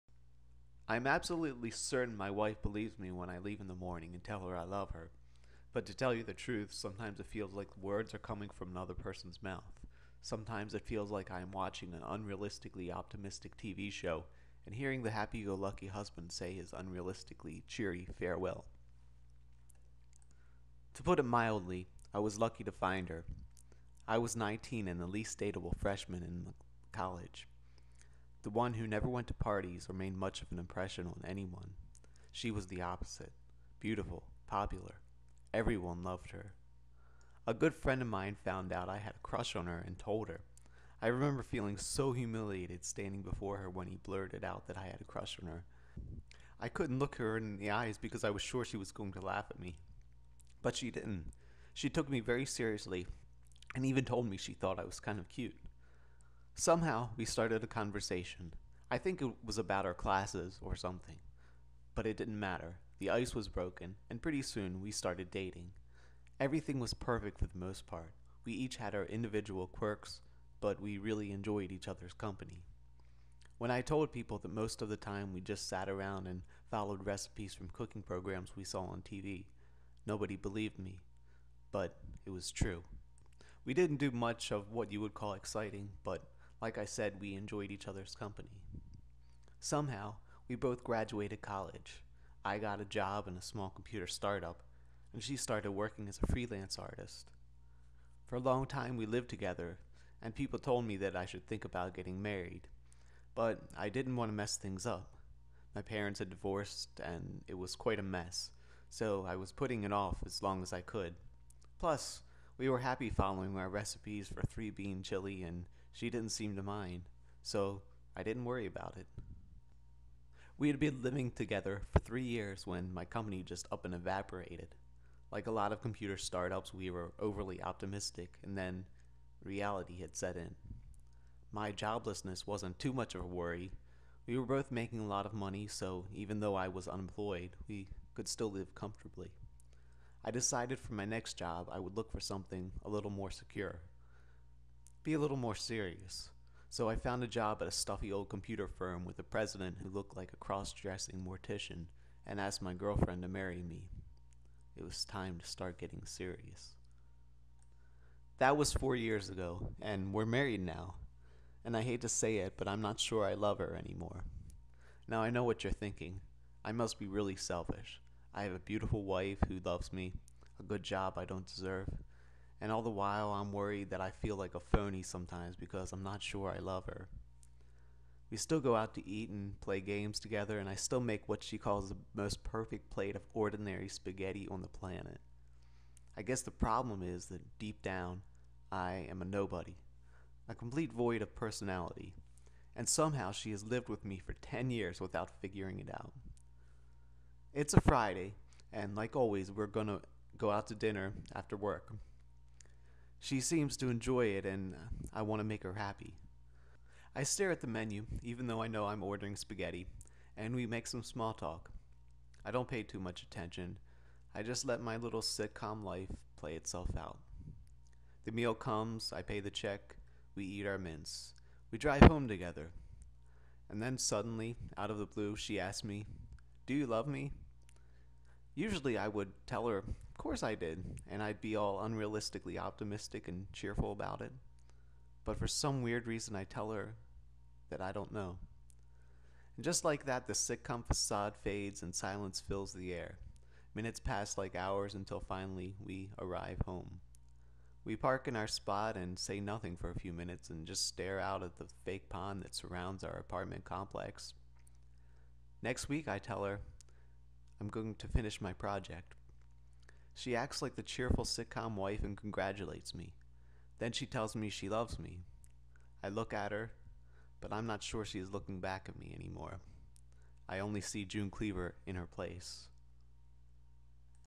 Happy-Go-Lucky
A short story about a sitcom life.

I wrote this story a few years ago for a local magazine. It was my attempt at a Haruki Murakami style story.